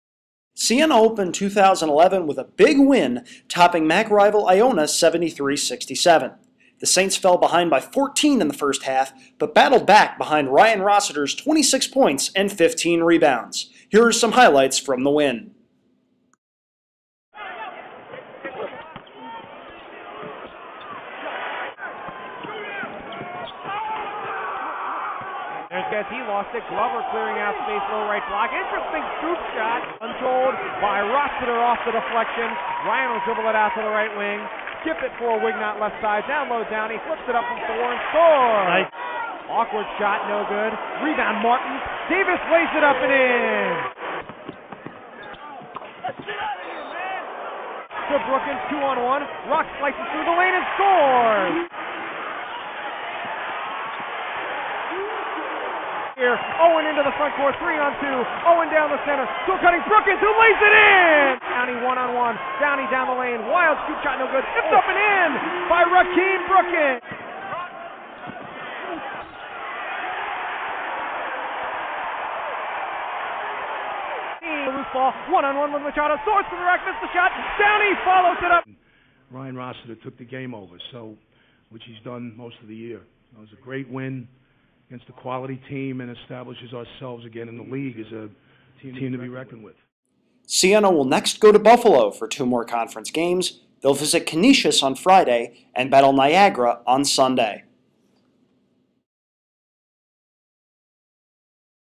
Siena-Iona Highlights
See all the highlights from Siena's come-from-behind victory over Iona, and hear Mitch Buonaguro's thoughts on the 1,000th win in Siena men's basketball history!